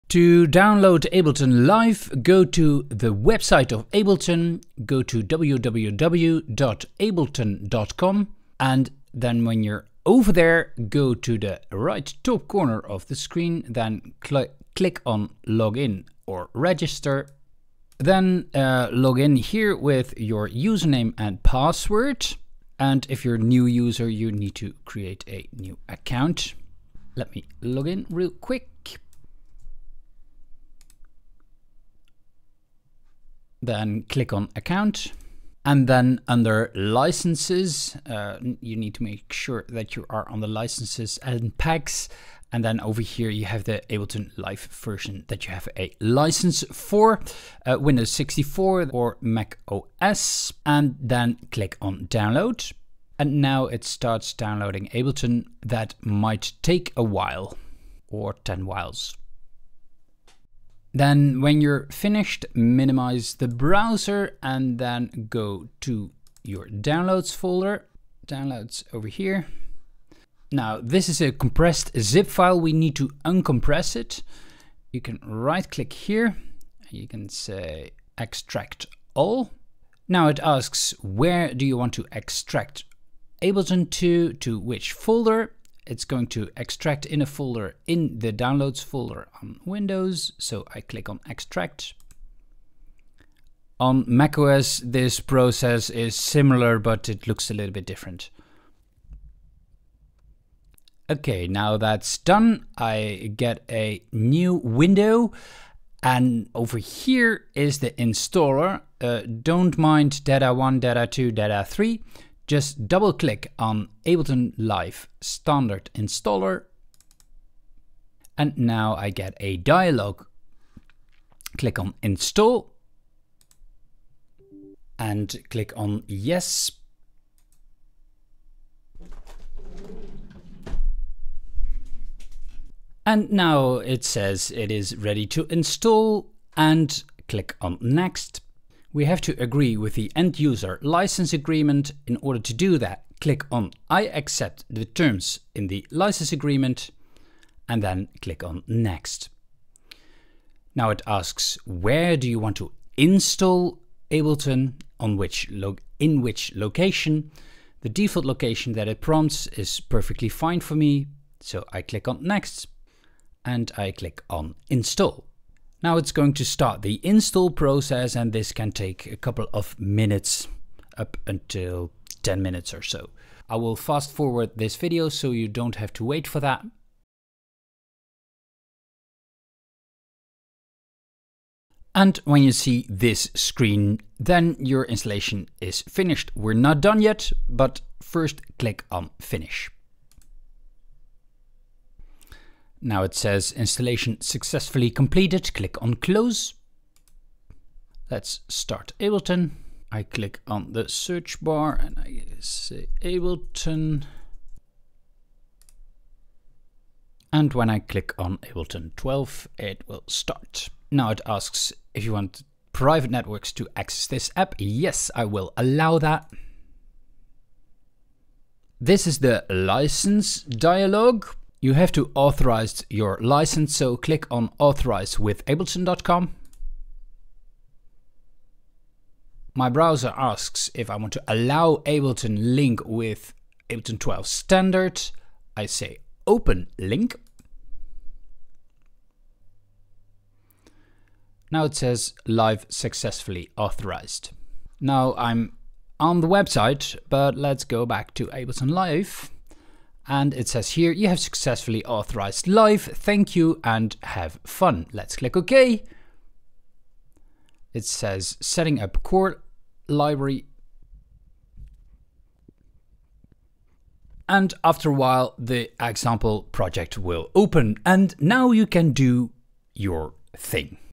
Install Ableton Live 12 on Windows - 2026 - Intro / Standard / Suite
How to install Ableton Live on Windows: Intro, Suite and Standard

• 0:00 Download Ableton
• 1:10 Install Ableton
• 3:17 Authorize Ableton